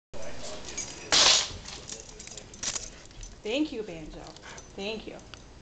Banjo picking up keys
Dropped my keys and Banjo picks them up. He is in the early stages of training.